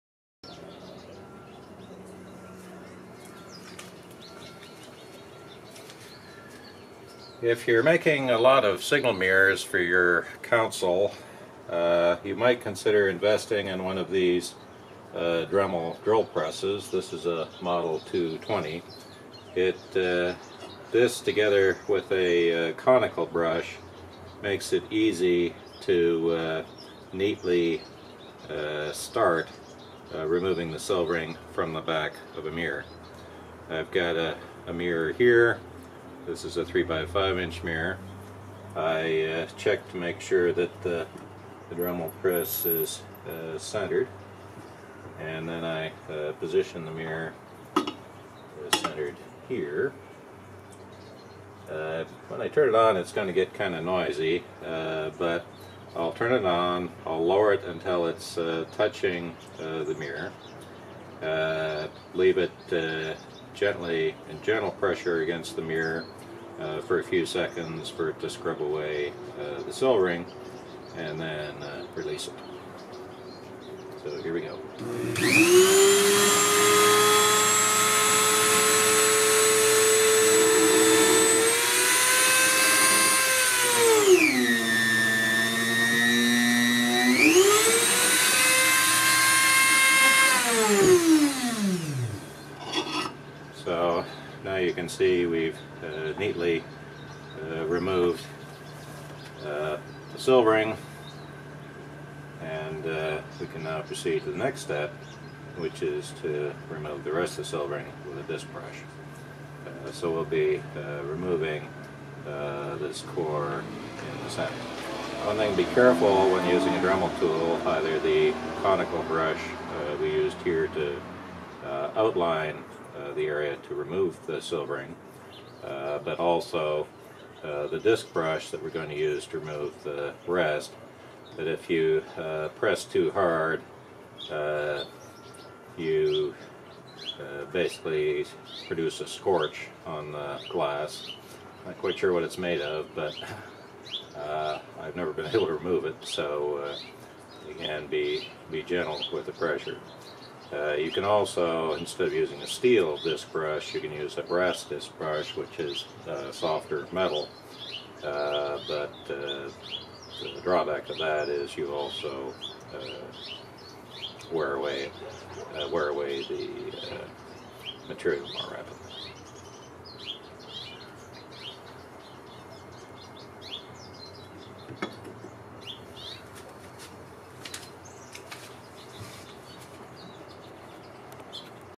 Using the Dremel 220 Rotary Workstation to start a signal mirror aimer
This video demonstrates the use of a Dremel 220 Rotary Workstation (sort of a drill press for Dremel tools) with a Dremel tool and a conical steel brush to make a neat circular outline in the silvering in the back of a mirror as a preparatory step to removing a quarter-sized section of silvering to add a retroreflective tape aimer to the mirror. You probably only want to do this if you are making lots of signal mirrors, since the workstation itself, absent the Dremel tool, will set you back about $40. To see the following steps in making a signal mirror, see this video